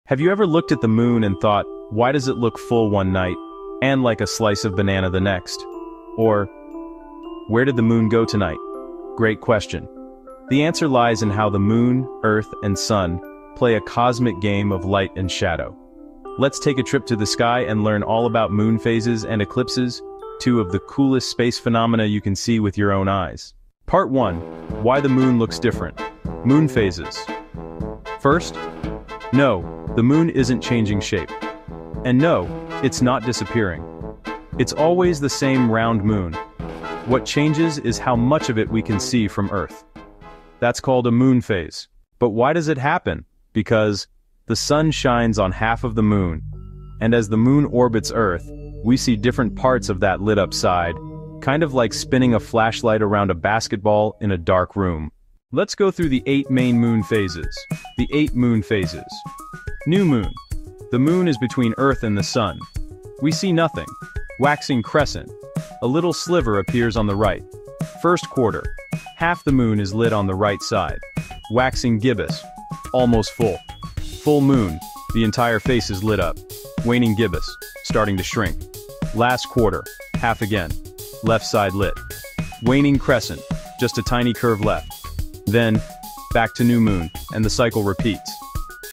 Moon Phases Explained: Why the Moon Changes Shape!
Ever wondered why the moon looks different each night? It's not changing shape or disappearing—it's all about how sunlight reflects as the moon orbits Earth. Let's explore moon phases!